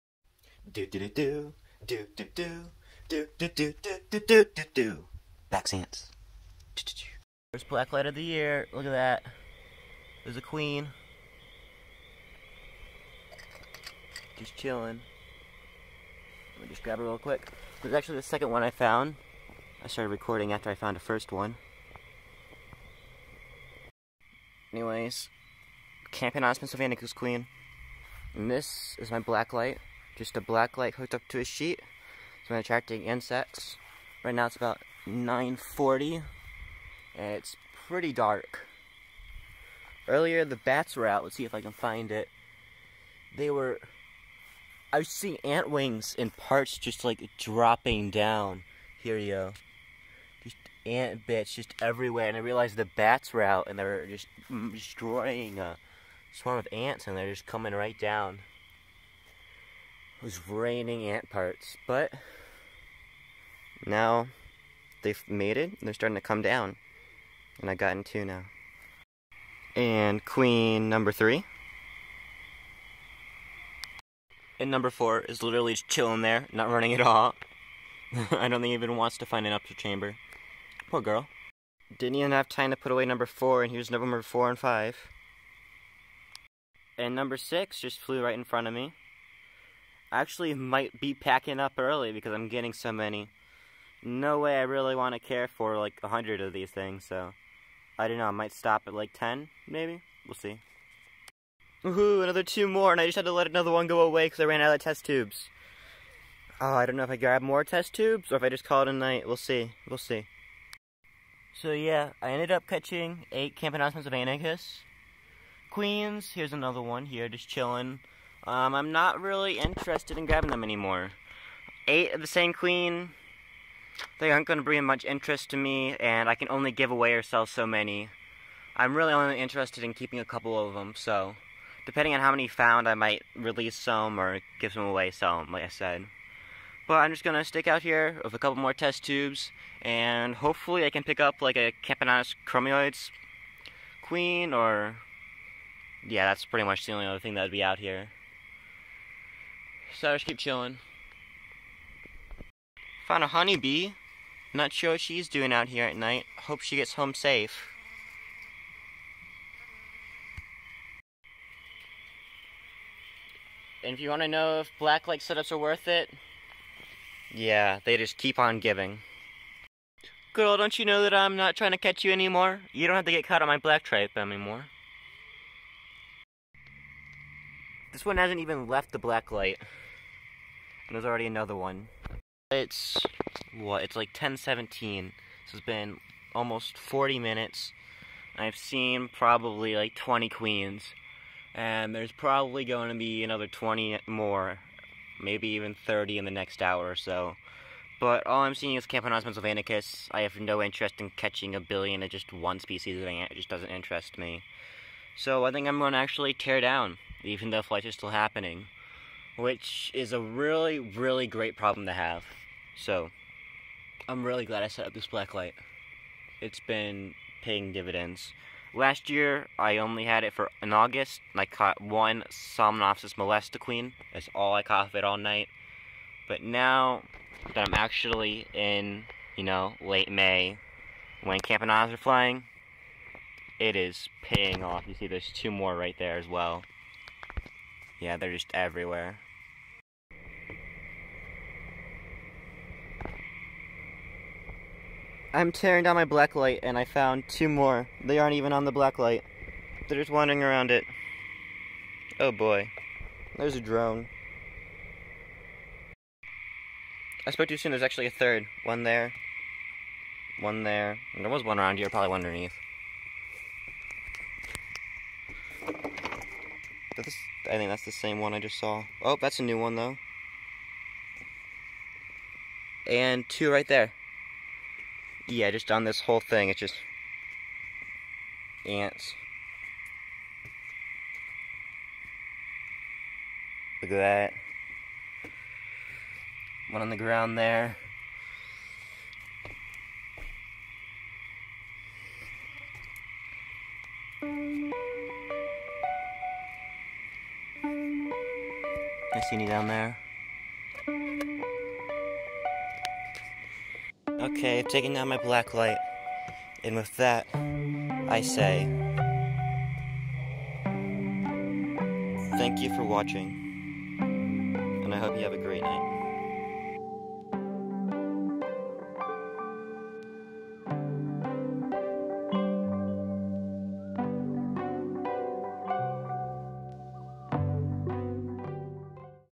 Night Time Black-lighting [Looking for Ant Queens]
In this video I set up a Black-Light trap and enjoy the night catching Camponotus pennsylvanicus (Carpenter Ant) queens. Video recorded on May 25th in around 75 degree weather

Here is a guide from Ant Dorks on how to make a Blacklight setup

Also Make sure to visit OhioAnts.com